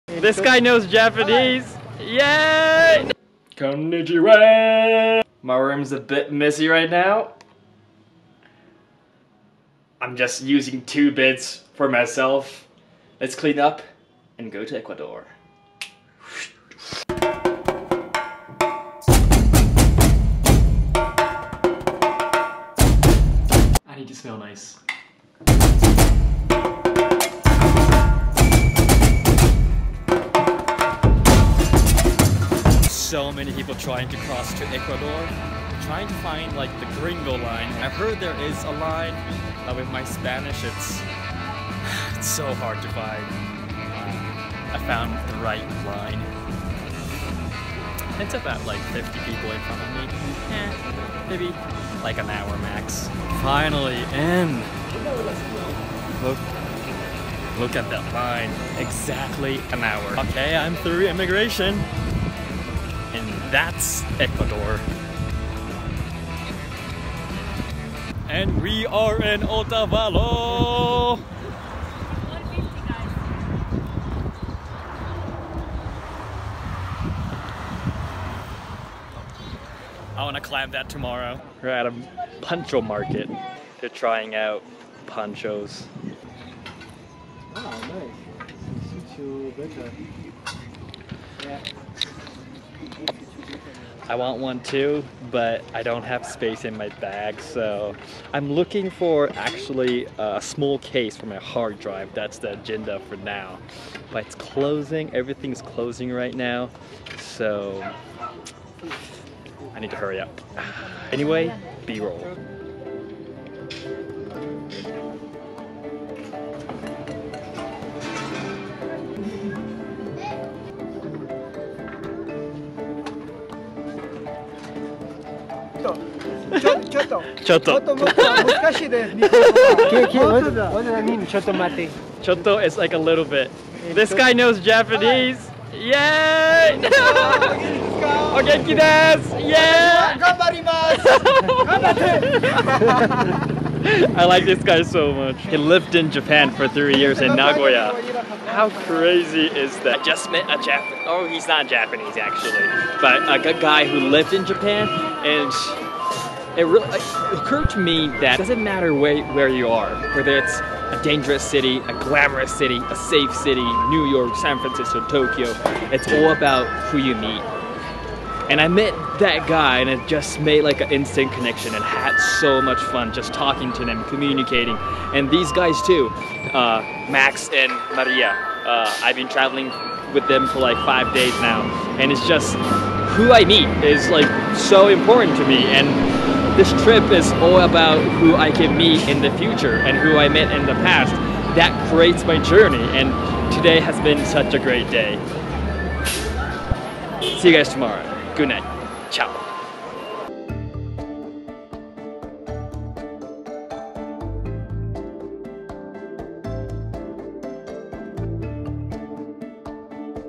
First Day in Ecuador Vlog. Day 20
After going through the scary immigration process, I eventually met up with Max & Maria. Otavalo was our next destination. After a 4 hour bus ride, we arrived there and we first went to the puncho market. There I met a guy who spoke a little bit of Japanese and it was an instant bond. It was a great start to Ecuador.